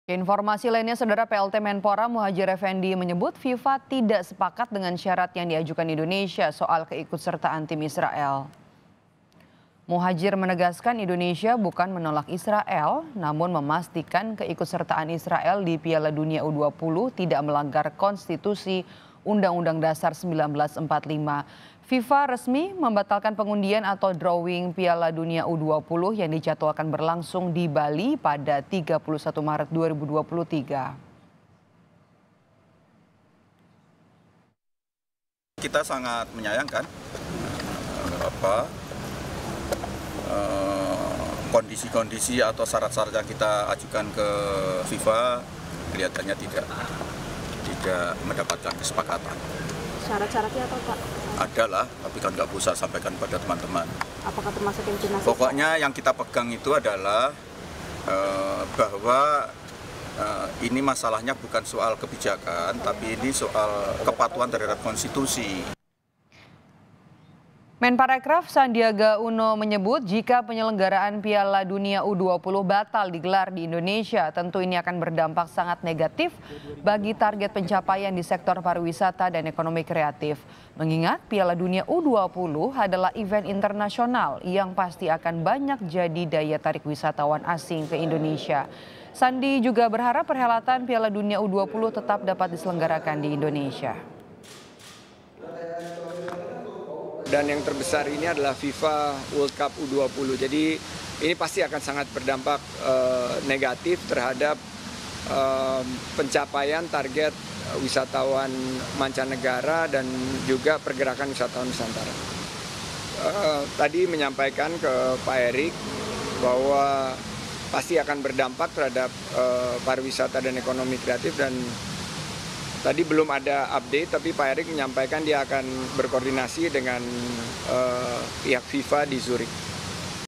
Cari Solusi Penyelenggaran Piala Dunia U20, Ini Kata PLT Menpora Muhadjir Effendy!
JAKARTA, KOMPAS.TV - PLT Menpora, Muhadjir Effendy sebut FIFA tidak sepakat dengan syarat yang diajukan Indonesia, terkait keiukutsertaan tim Israel.

Muhadjir menegaskan, Indonesia bukan menolak Israel namun memastikan keikutsertaan Israel di Piala Dunia U20, tidak melanggar konstitusi UUD 1945.

FIFA resmi batalkan pengundian atau drawing Piala Dunia U20, yang dijadwalkan berlangsung di Bali, pada (31/03).

Sementara itu, Menparekraf Sandiaga Uno menyatakan, jika penyelenggaraan Piala Dunia U20 batal digelar di Indonesia, tentu ini akan berdampak sangat negatif bagi target pencapaian di sektor pariwisata dan ekonomi kreatif.

Mengingat Piala Dunia U20 adalah event internasional yang pasti akan banyak jadi daya tarik wisatawan asing ke Indonesia.

Sandi berharap, perhelatan Piala Dunia U20 tetap dapat diselenggarakan di Indonesia.